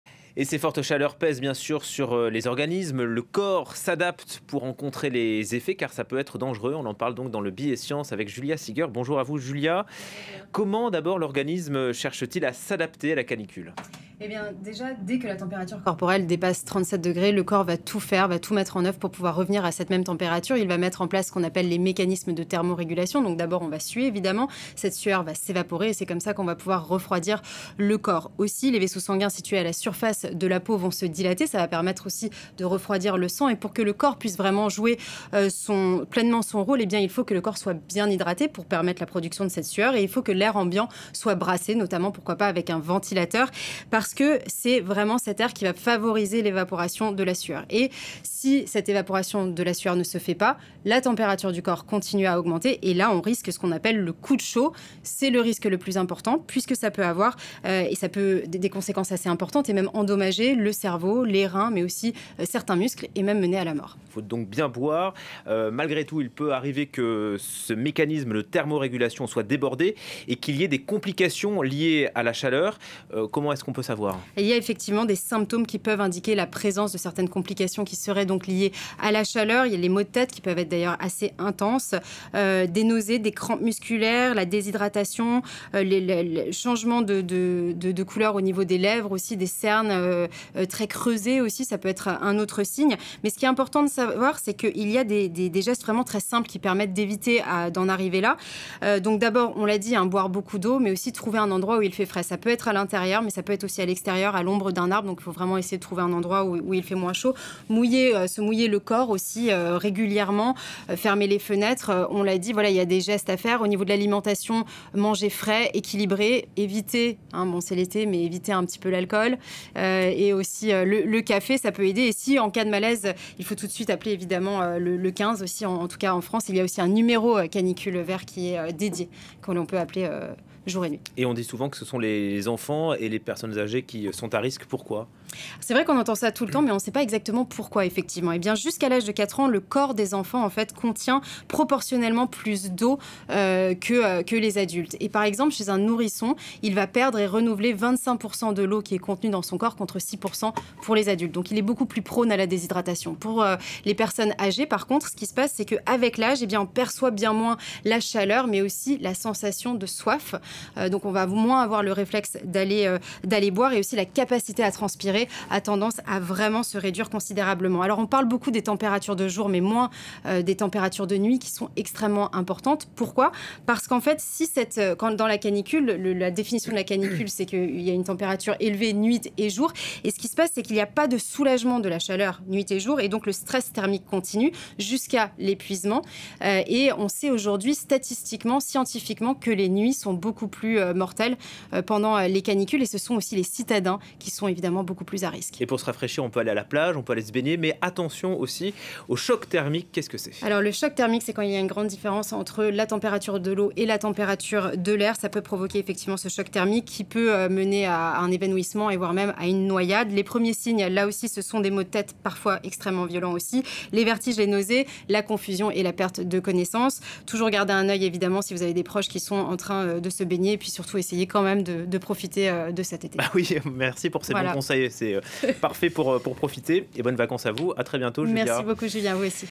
Vagues de chaleur : comment l’organisme s’adapte-t-il à la canicule ? • FRANCE 24
Les fortes chaleurs de ces derniers jours ne laissent pas le corps indifférent. Entre maux de tête, déshydratation ou nausées, les effets de la canicule peuvent être dangereux pour la santé. Comment notre organisme s’adapte-t-il face à ce phénomène ? Julia Sieger nous apporte des éléments de réponse dans la chronique science du jour.